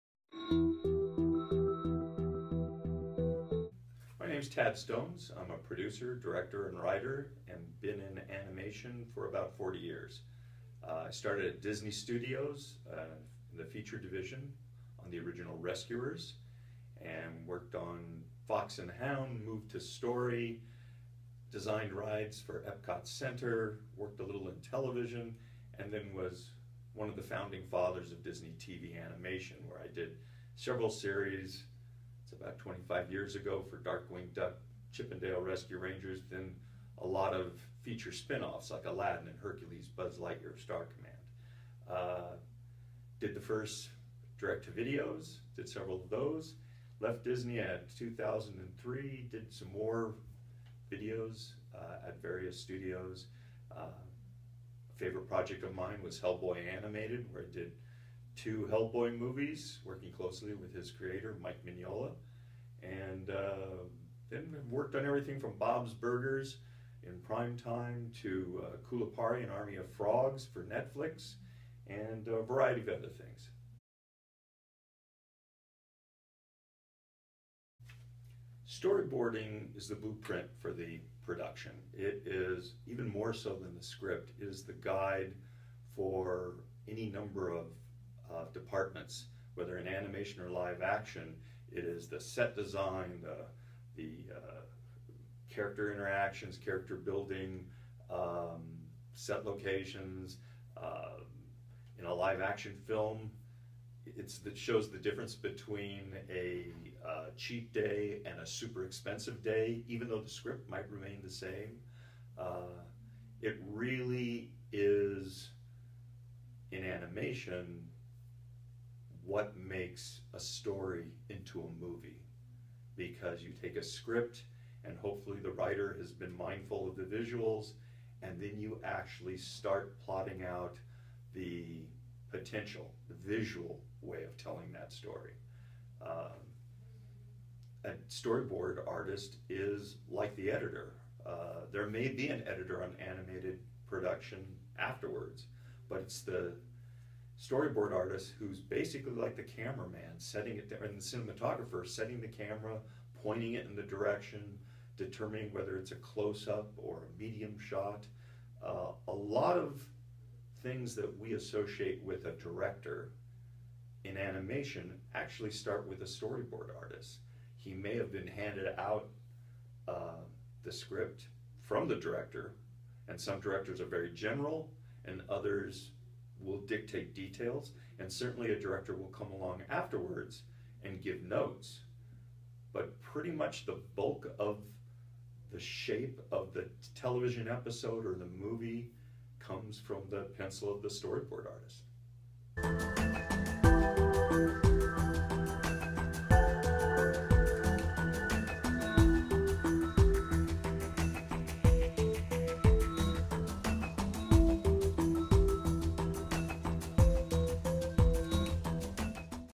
Storyboarding tips storyboardingbook Tad Stones A
Storyboarding tips  storyboardingbook

Tad Stones is a Producer/Writer/Artist who started his animation career at Walt Disney Features then moved to Disney Imagineering to design rides for EPCOT Center. He was one of the founders of Walt Disney TV Animation where he created such shows as Chip ’n’ Dale’s Rescue Rangers and Darkwing Duck along with several feature spinoffs.  After 30 years at Disney, Tad produced several direct to video features, most notably two Hellboy Animated movies, Hellboy: Sword of Storms and Hellboy: Blood and Iron. He he also cowrote and directed the Netflix Original Series, Kulipari: An Army of Frogs